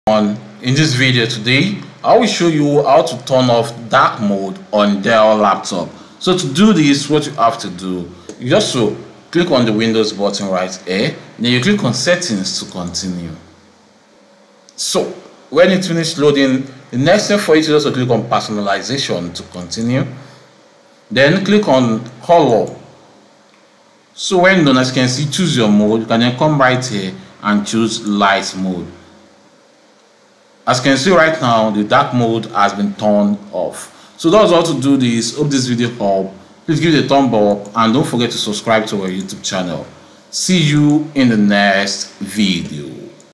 How to Turn Off Dark Mode on Dell Laptop
In this video, I will show you How to Turn Off Dark Mode on Dell Laptop.